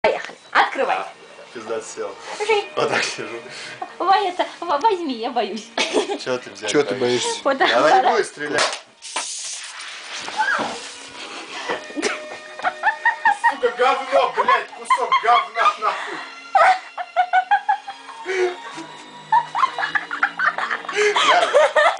КАК ОТКРЫВАТЬ ШАМПАНСКОЕ? ОТКРЫЛ БУТЫЛКУ ШАМПАНСКОГО. РЖАЧ.
Как открыть шампанское? Кто не рискует, тот не пьет шампанское. Открыл бутылку. Вова очень "аккуратно" открыл бутылку дорогого шампанского .
Смотри видео /♥ Ставь Like ♥/This is Me ! /♥Like and Follow ♥

    Ваша Машенька)) !!